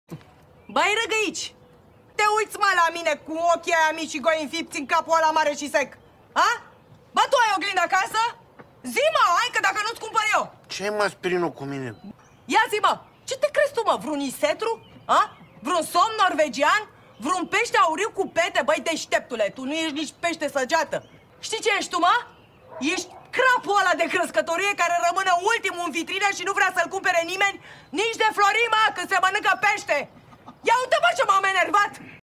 Pinguinii - Doamna si Vagabondul 🐧👈🤣👉 🐧 Penguins - Lady and the Tramp
Doamna si Vagabondul 🐧 - Cum incerci sa cuceresti, dar nu-ti iese! 🤣
Lady and the Tramp 🐧 - How you try to conquer but it doesn't work out! 🤣
This video is subtitled in english!!